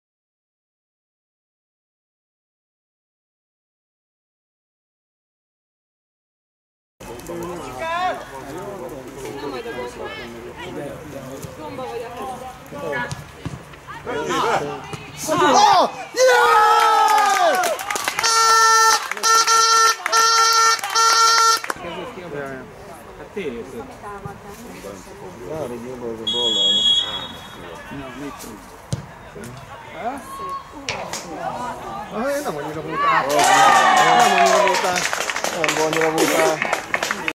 U15: KTE LA - Győr 1-1
U15: KTE LA – Győr 1-1 (1-1)

U15 I. osztály, 8. forduló

KTE: Gyapjas – Barkóczi, Szél, Heidrich (Bogdán 39.), Horváth Á. – Szalai, Győri, Kovács Á. – Lengyel, Goretich, Molnár (Pető 68.).

Gól: Győri (14.) ill. Molnár K. (34.)